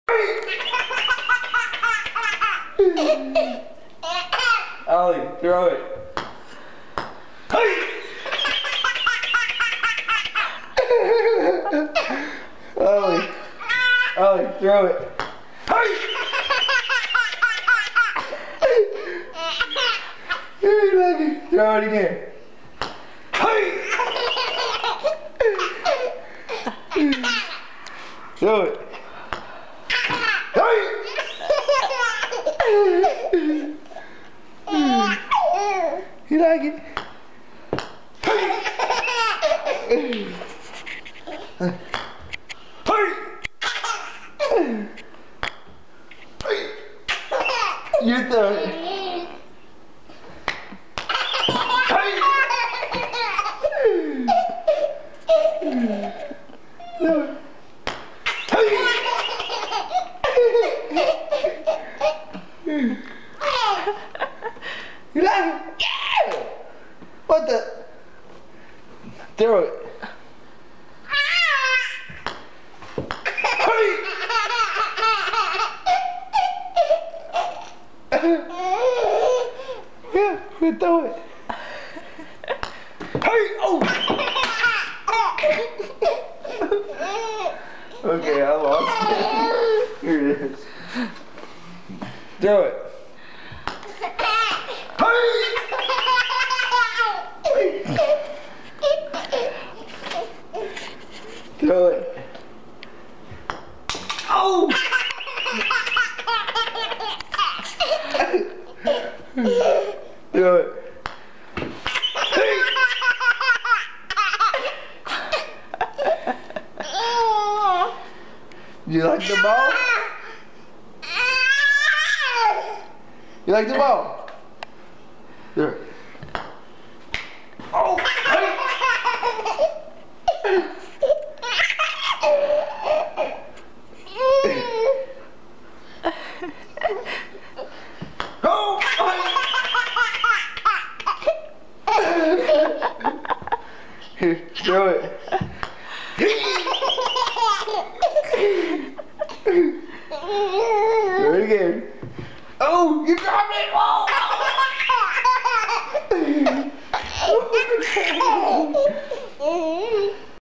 Baby Laughing Hysterically At Dad
Little Oliver laughing at his dads vanishing golf ball trick!!
Please hit "LIKE" if you enjoyed our video!